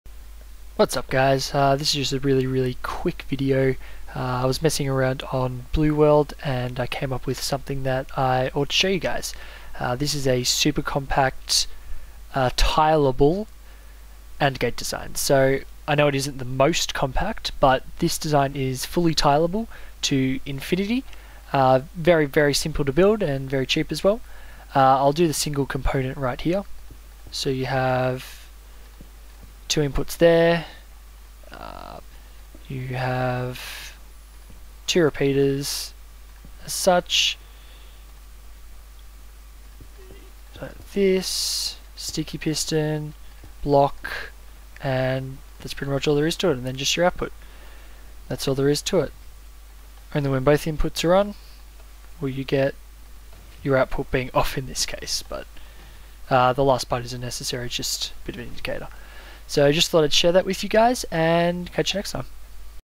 Super-Compact Tileable AND Gate
Just a super-quick video showing something I made while mucking arouind in Blue World.

Dimensions: 3x3x1, more if you include the input/output lines.